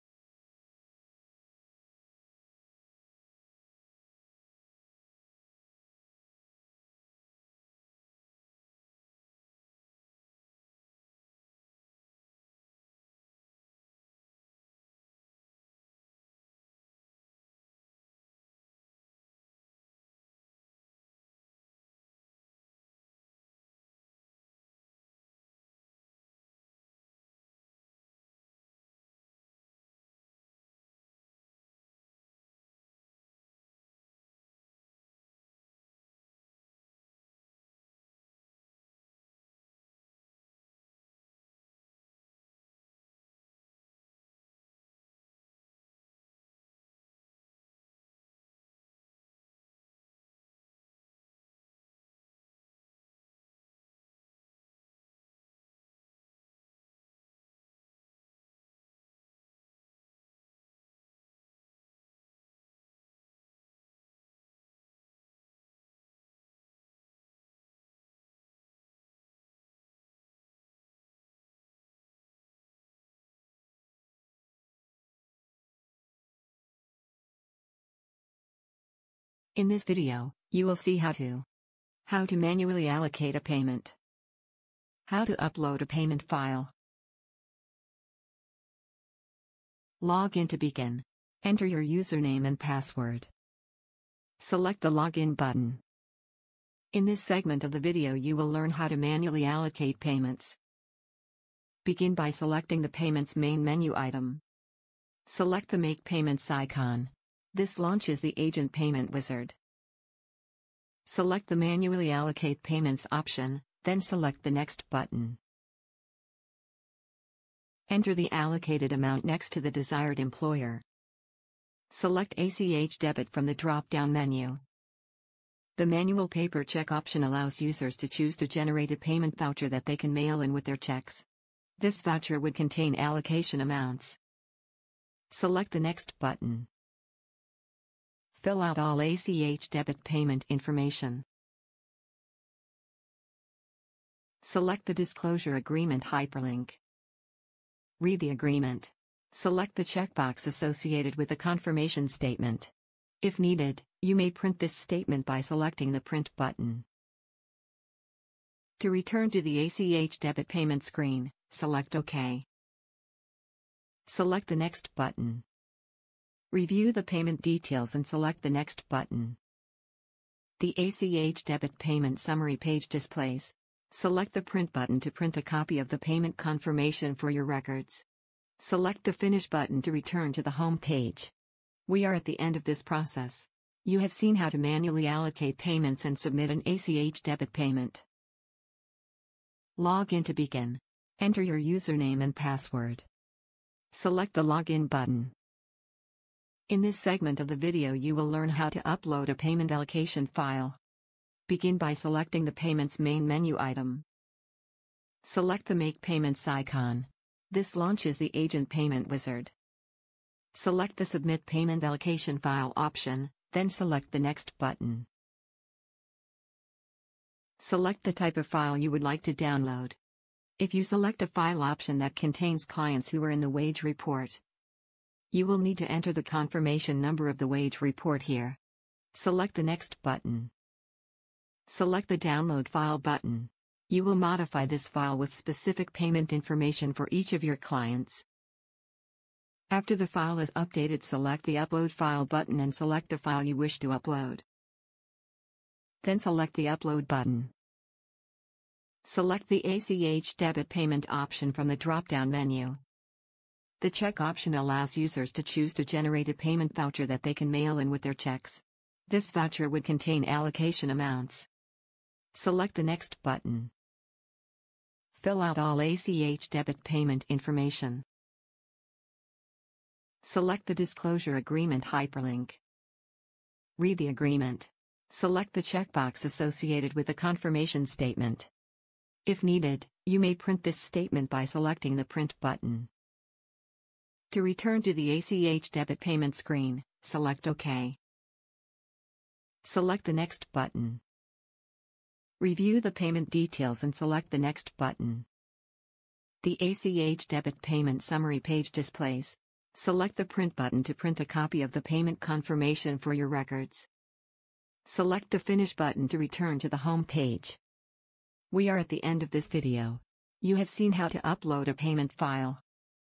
Maryland Unemployment Insurance BEACON Third Party Payment Submission
This video shows agents how to manually allocate payment and upload a payment file in the BEACON 2.0 system.